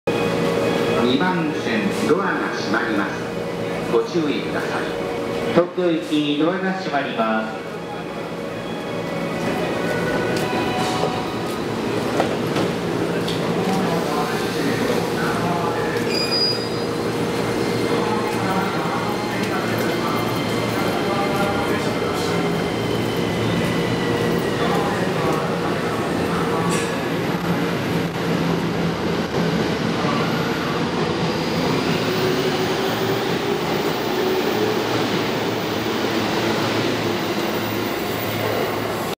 【JR東日本】E231系近郊型未更新車JR東海道本線新橋駅発車(UTL開業前)
[普通]東京